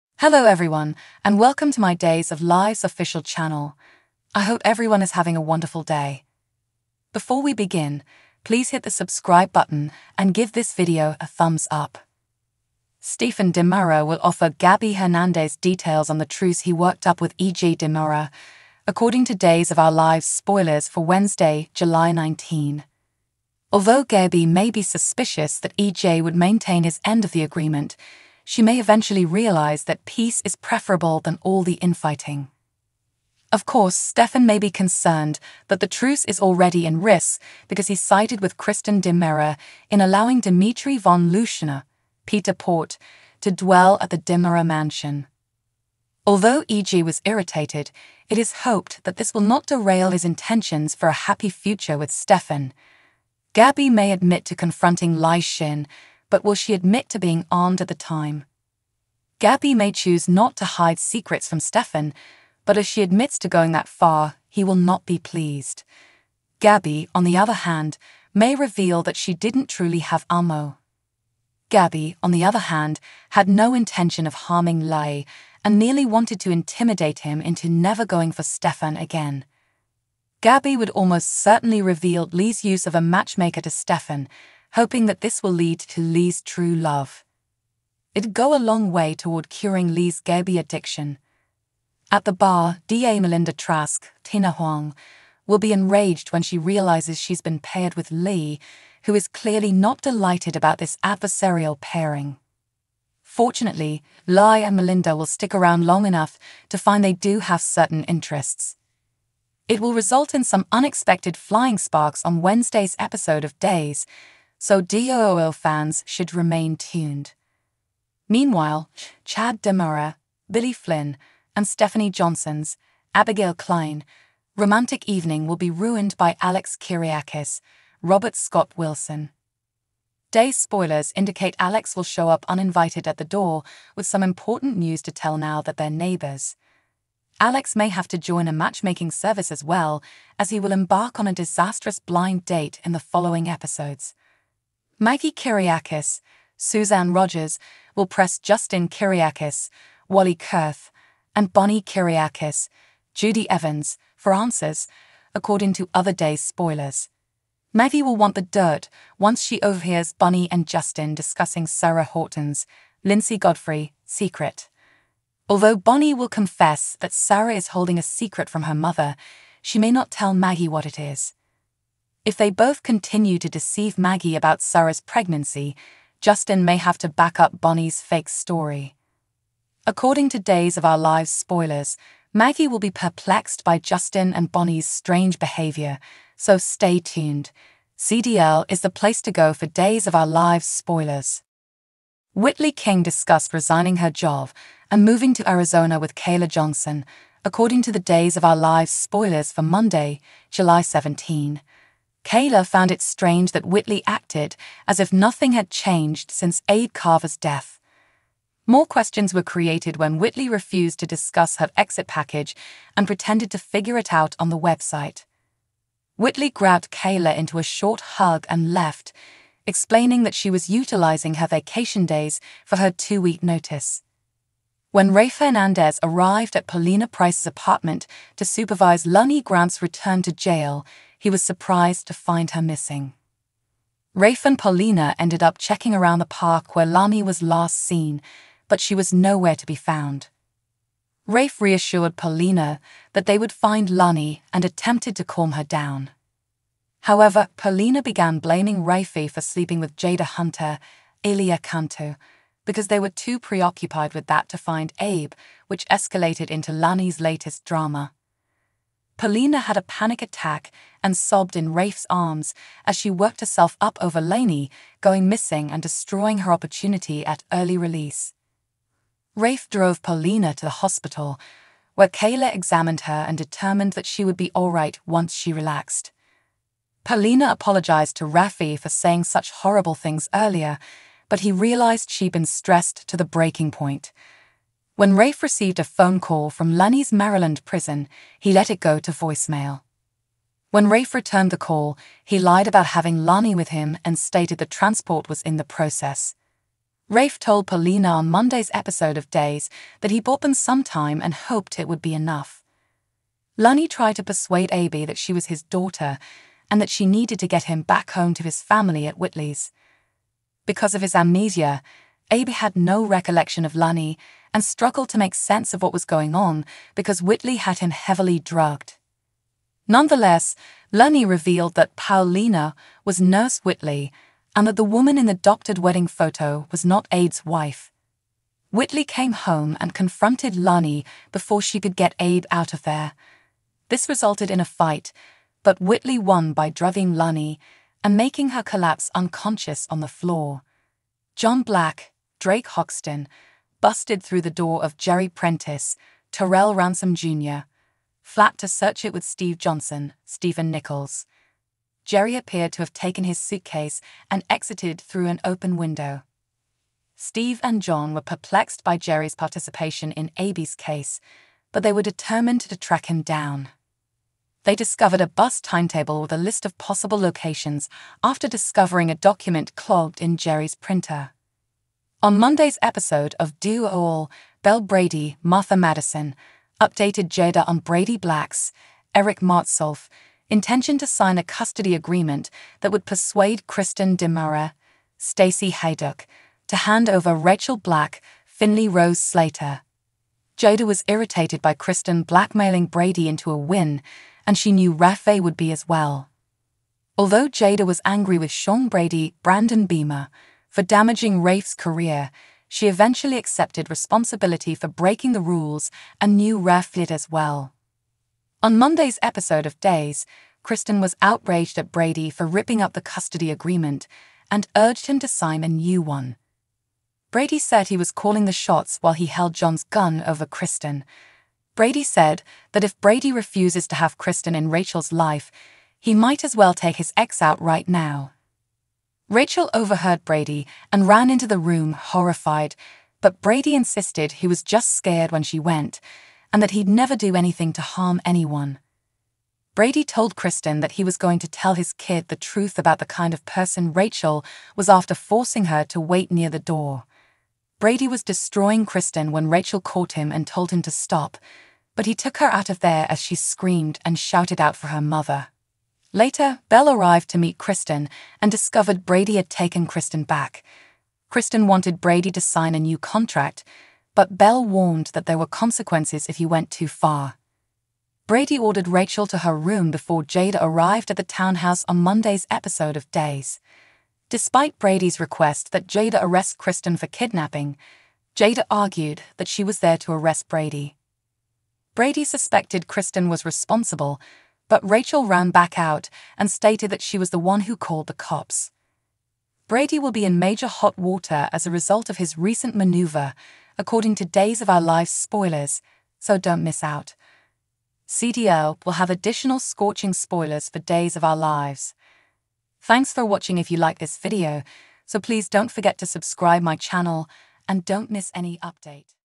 "Drama Unleashed: Bonnie Makes a Confession That Rocks Salem - Must-See Revelation!".
Bonnie's Bombshell Confession to Maggie - Salem's Drama Intensifies!"
"Melinda's Shocking Discovery: Li's Her Date? The Unlikely Connection Revealed!"
"Salem's Secrets Unveiled: Bonnie Buckles Under Pressure - The Truth Comes Out!"
"Melinda's Horror: Discovering Li's True Identity - Salem's Newest Twist Exposed!"